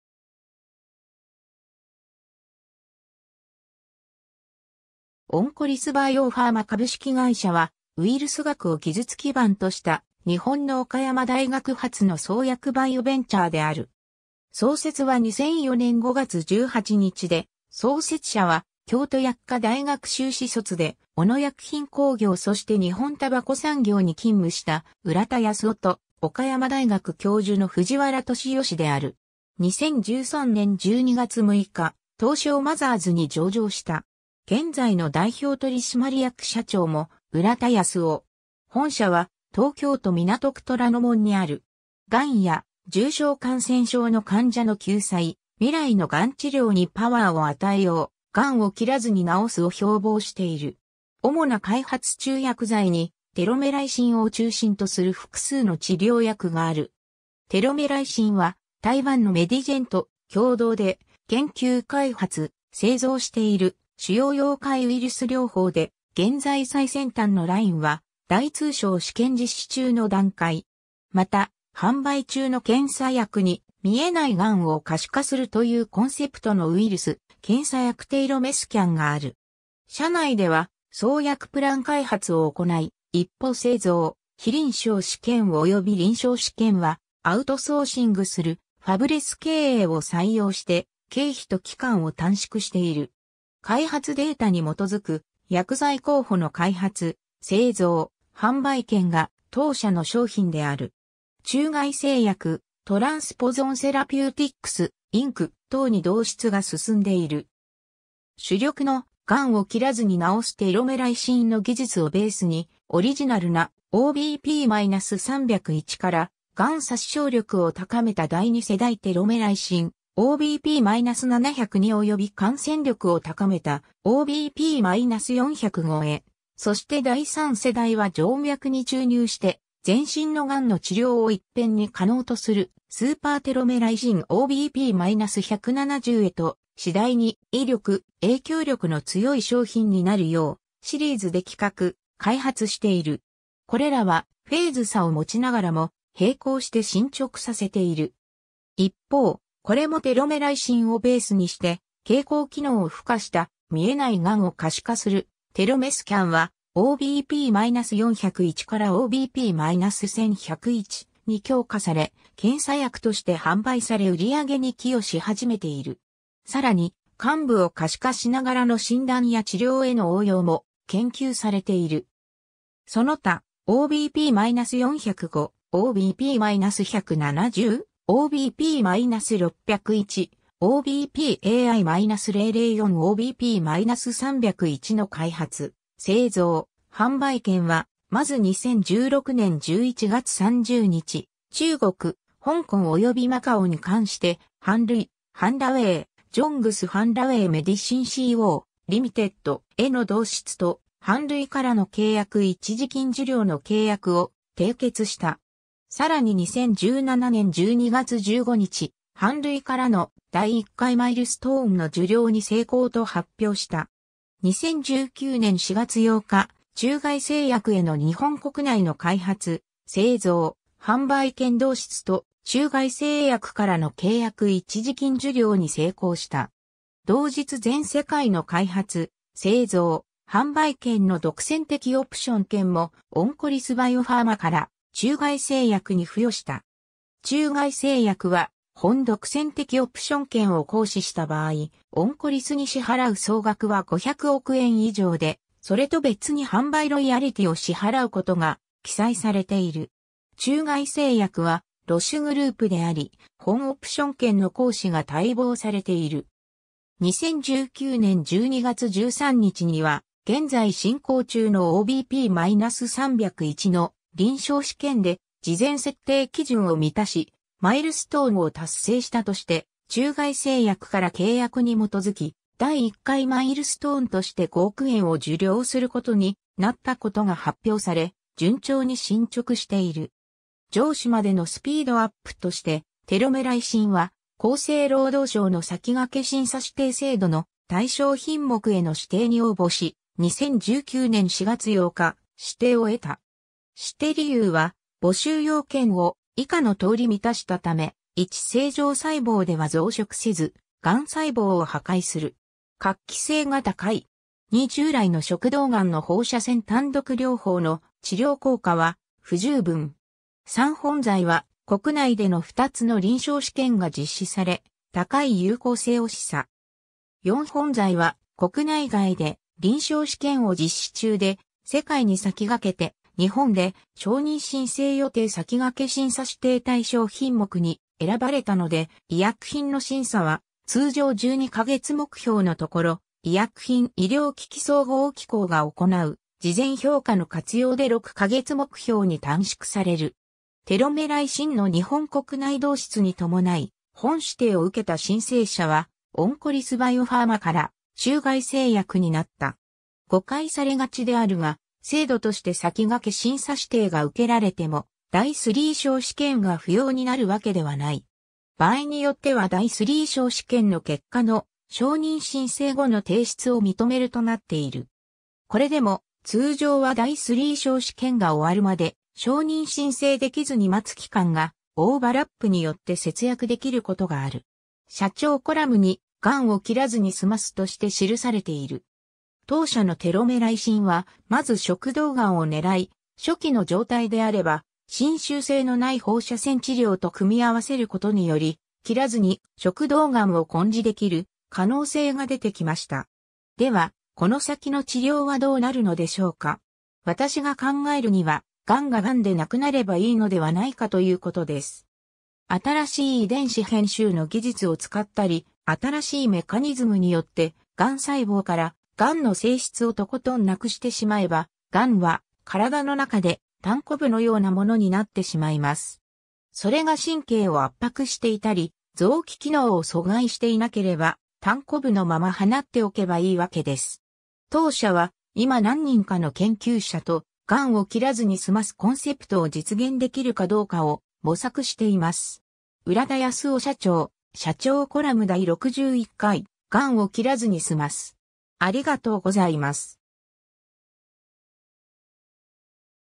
オンコリスバイオファーマ
オンコリスバイオファーマ株式会社（英：Oncolys BioPharma Inc.）は、ウイルス学を技術基盤とした日本の岡山大学発の創薬バイオベンチャーである。
 創設は2004年5月18日で、創設者は、京都薬科大学修士卒で小野薬品工業そして日本たばこ産業に勤務した浦田泰生と、岡山大学教授の藤原俊義である。
 2013年12月6日、東証マザーズに上場した。
現在の代表取締役社長も浦田泰生。
本社は東京都港区虎ノ門にある。
 がんや重症感染症の患者の救済、「未来のがん治療にパワーを与えよう（Powering Future Oncotherapy）」、「がんを切らずに治す」を標榜している。
 主な開発中（治験中含む）薬剤に「テロメライシン」を中心とする複数の治療薬がある。
テロメライシン (OBP-301) は台湾のメディジェン（基亞生物科技股份有限公司）（Medigen Biotechnology Corp.）と共同で研究開発・製造している腫瘍溶解ウイルス療法で、現在最先端のラインは第II相試験実施中の段階。
 また、販売中の検査薬に「見えないがんを可視化する」というコンセプトのウイルス検査薬テロメスキャンがある。
 社内では創薬プラン開発を行い、一方製造、非臨床試験および臨床試験はアウトソーシングするファブレス経営を採用して経費と期間を短縮している。
開発データに基づく、薬剤候補の開発・製造・販売権が当社の商品である。
中外製薬、Transposon Therapeutics, Inc.等に導出が進んでいる（別項#開発・製造・販売権の導出）。
 　主力の「がんを切らずに治す」テロメライシンの技術をベースに、オリジナルな OBP-301 からがん殺傷力を高めた第二世代テロメライシン OBP-702 および感染力を高めた OBP-405へ、そして第三世代は静脈に注入して全身のがんの治療をいっぺんに可能とするスーパーテロメライシン OBP-170□ へと、次第に威力、影響力の強い商品になるよう、シリーズで企画・開発している。
これらはフェーズ差をもちながらも、並行して進捗させている。
 一方、これもテロメライシンをベースにして蛍光機能を付加した「見えないがんを可視化する」テロメスキャンは、OBP-401からOBP-1101に強化され、検査薬として販売され売上に寄与しはじめている。
さらに、患部を可視化しながらの診断や治療への応用も研究されている。
 　その他：OBP-405（Telomelysin-RGD）（別名：次世代テロメライシン、第2世代テロメライシン）、OBP-170□（スーパーテロメライシン）、OBP-601（センサブジン; Censavudine）、OBP-AI-004 OBP-301の開発・製造・販売権は、まず2016年11月30日、中国、香港およびマカオに関してハンルイ（江苏恒瑞医药股份有限公司）（略称：恒瑞）(Hengrui ; Jiangsu Hengrui Medicine Co., Ltd.）への導出（ライセンスアウト）とハンルイからの契約一時金受領（金額非開示）の契約を締結した。
さらに2017年12月15日、ハンルイからの第一回マイルストーン（開発進展に応じた報酬）の受領（金額非開示）に成功と発表した。
 2019年4月8日、中外製薬への日本国内の開発・製造・販売権導出と中外製薬からの契約一時金（5.5億円）受領に成功した。
同日（一部地域を除く）全世界の開発・製造・販売権の独占的オプション権もオンコリスバイオファーマから中外製薬に付与した。
中外製薬は本独占的オプション権を行使した場合、オンコリスに支払う総額は500億円以上で、それと別に販売ロイヤリティを支払うことが記載されている。
中外製薬はロシュグループであり、本オプション権の行使が待望されている。
 2019年12月13日には、現在進行中のOBP-301の臨床試験で事前設定基準を満たしマイルストーンを達成したとして、中外製薬から契約に基づき第1回マイルストーンとして５億円を受領することになったことが発表され、順調に進捗している。
 上市までのスピードアップとして、テロメライシン（OBP-301）は厚生労働省の先駆け審査指定制度 の対象品目への指定に応募し、2019年4月8日、指定を得た。
 指定理由は募集要件を以下のとおり満たしたた...